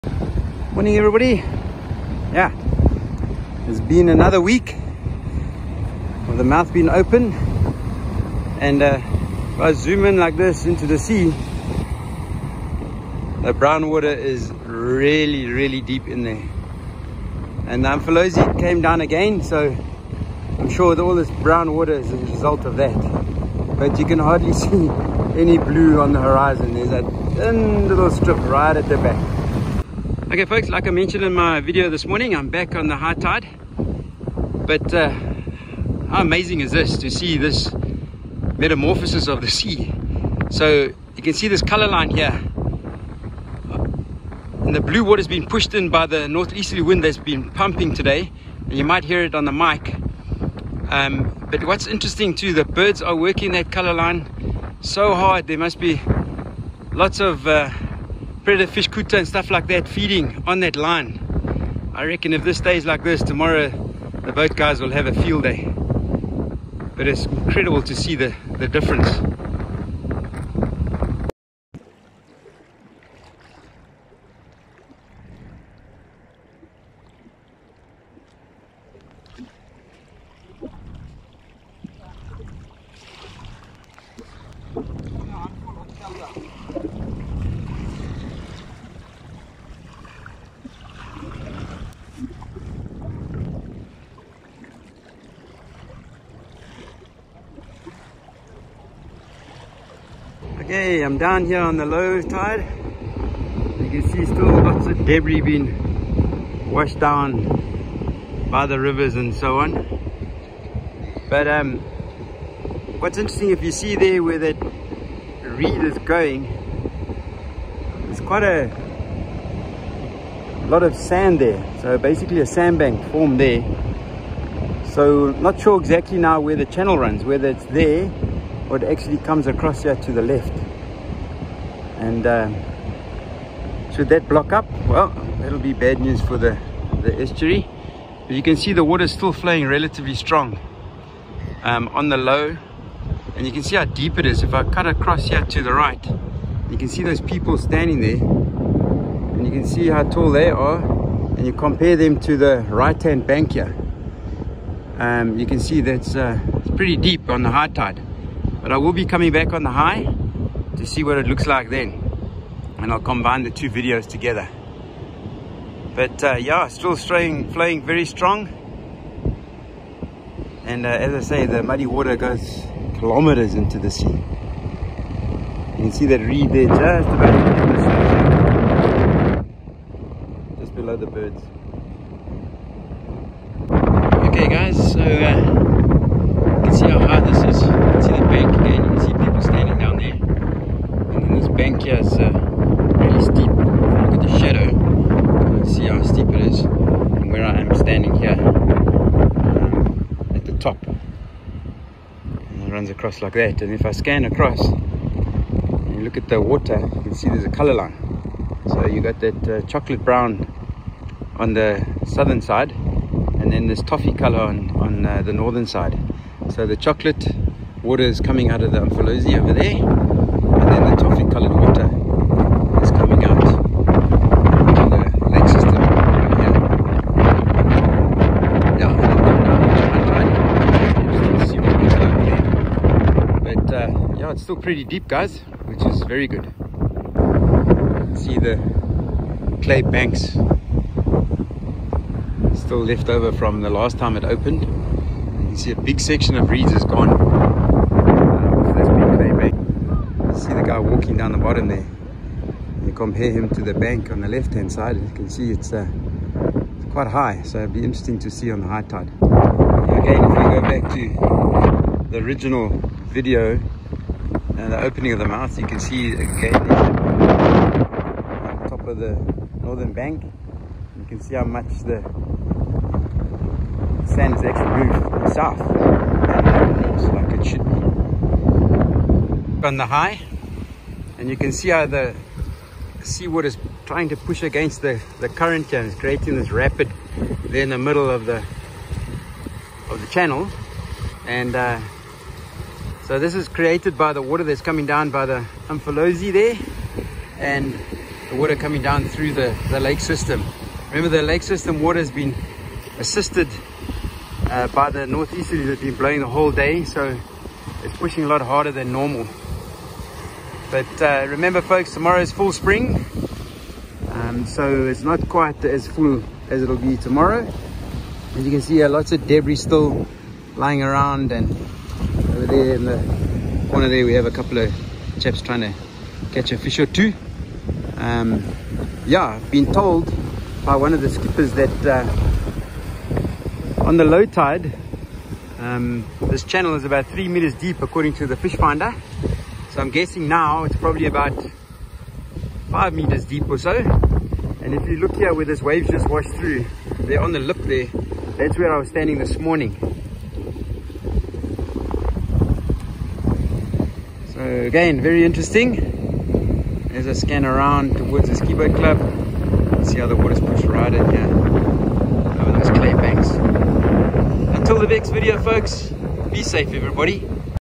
Estuary update 29/4/2022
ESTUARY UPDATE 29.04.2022

Video of todays Low and High Tide.

Maybe a marine biologist can help with the ID of the little crab’s